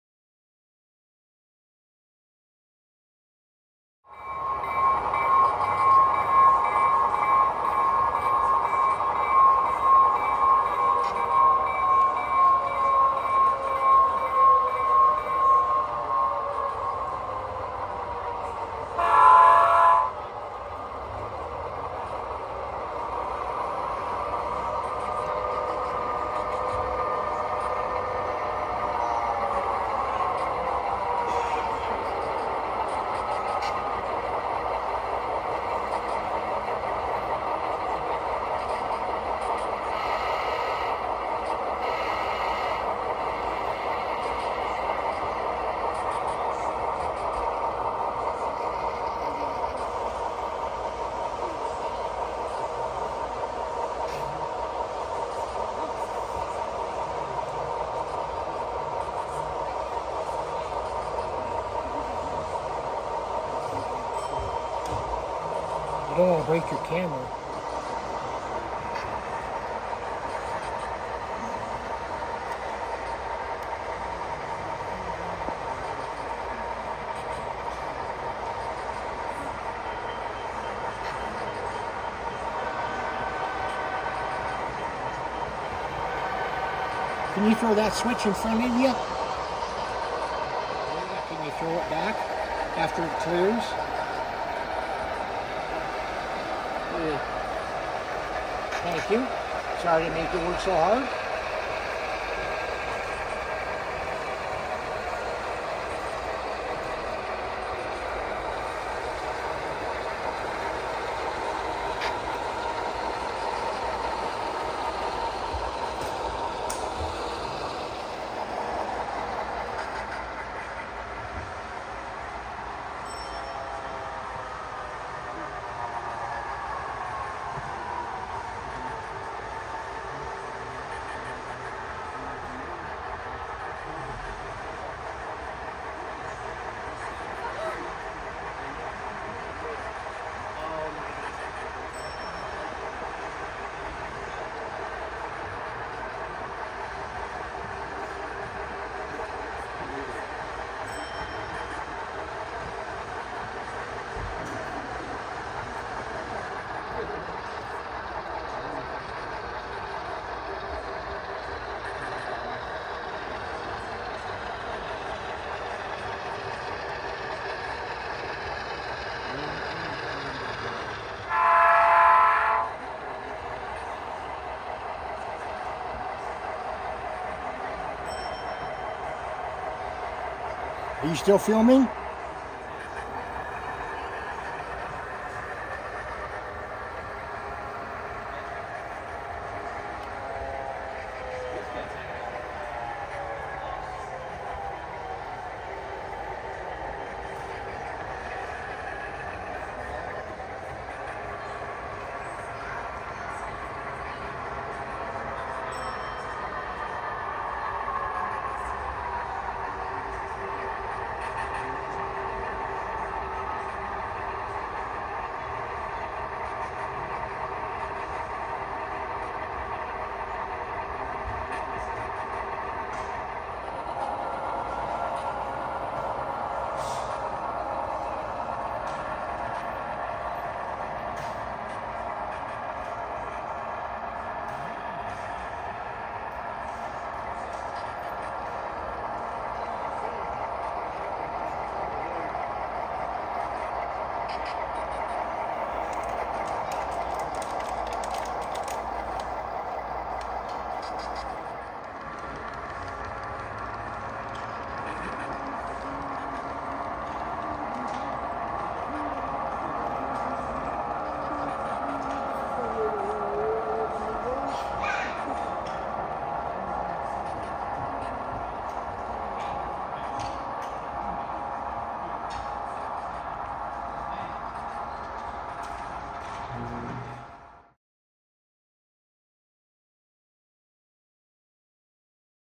BNSF Finishes His Switching
A look at the end of a switching job. At the end of the switching job, the volunteer had to back his train up to staging to change tracks to run in the layout.